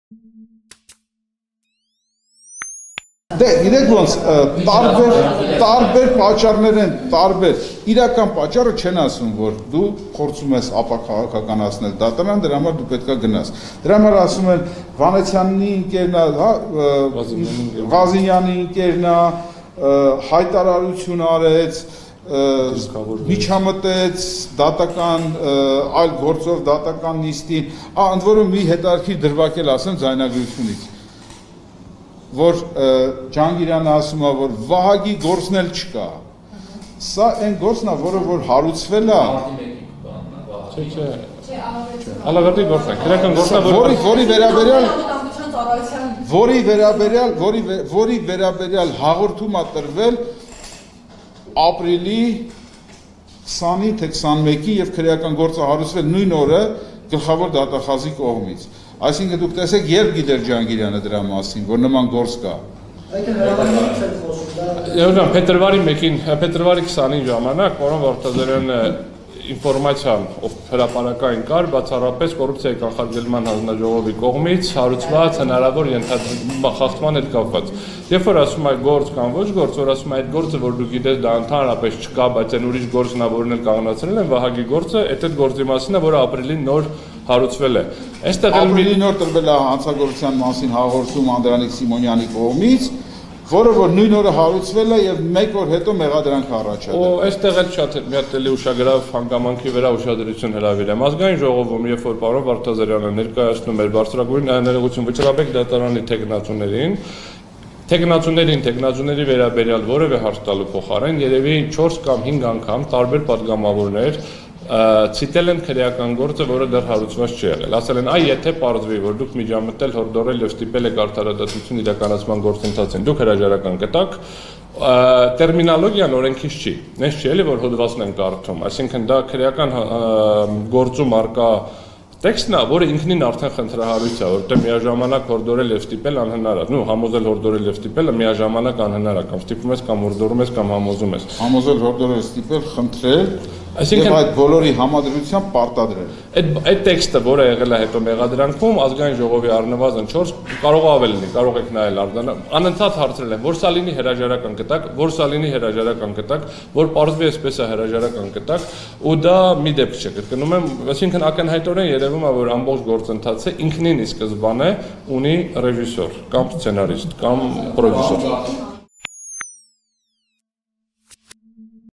Գագիկ Ջհանգիրյանն ու ՔՊ պատգամավորները նախապես իմացել են դեռևս չհարուցված քրգործի մասին
Ակնհայտ է, որ ամբողջ գործընթացը ունի ռեժիսոր և սցենարիստ. Փաստաբանը՝ աղմկահարույց ձայնագրության մասին